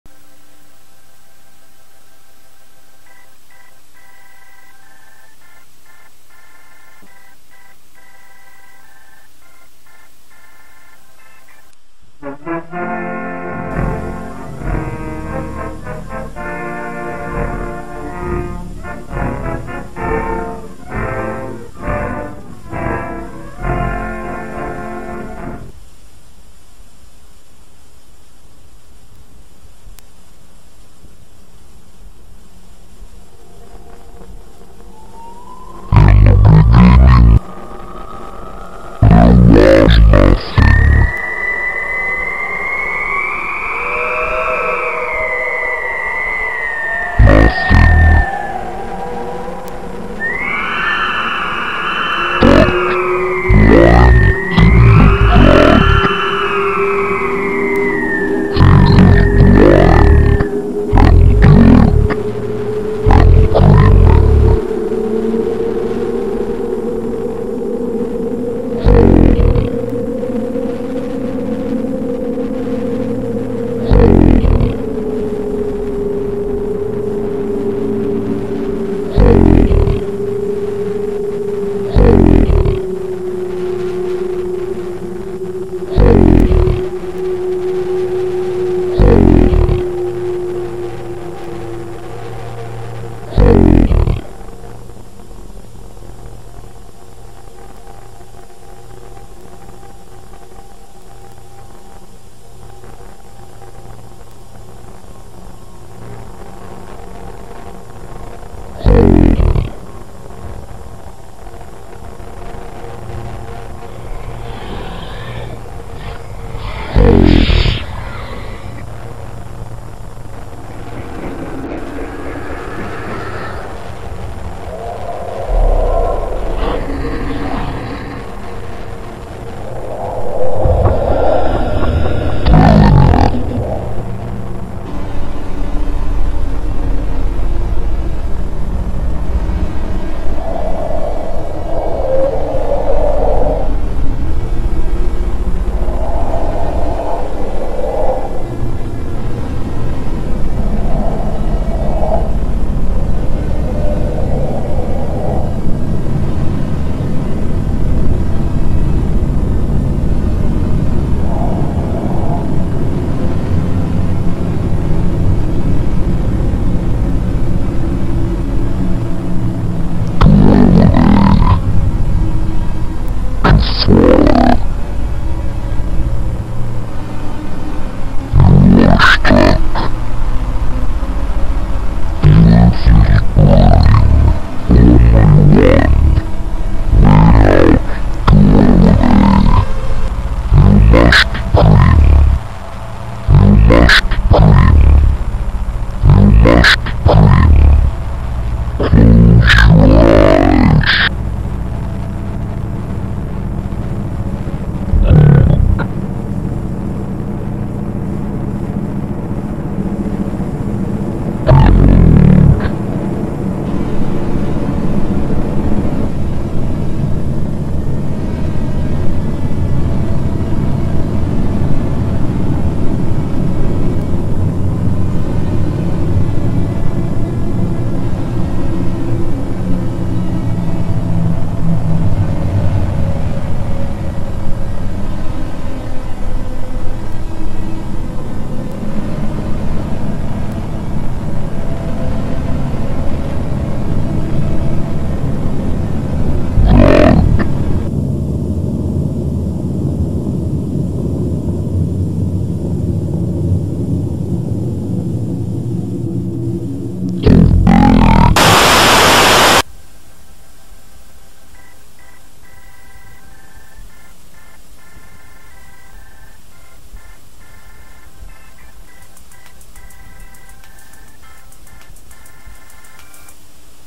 Your cosmology is wrong [Analog Horror]
It took me ages to dig this one out of the basement vault and restore it to viewable condition. EV102b seems to be similar to EV102a, in that it's another hijacked signal, probably from the early 90s. The imagery is disturbing.

The archive is full of old tapes. Most of them collecting dust or rotted out due to poor storage conditions.

Still no clues as to what happened to him, but hopefully there's something in here that can tell me.

Had to move into his place. The black vans parked outside my old apartment haven't followed me here, thankfully.

But now I'm having strange dreams. I feel like I'm being watched.

I'm working with a publisher to get the written transcripts out. They think they're just stories, but they're in for a shock. We all are, I fear.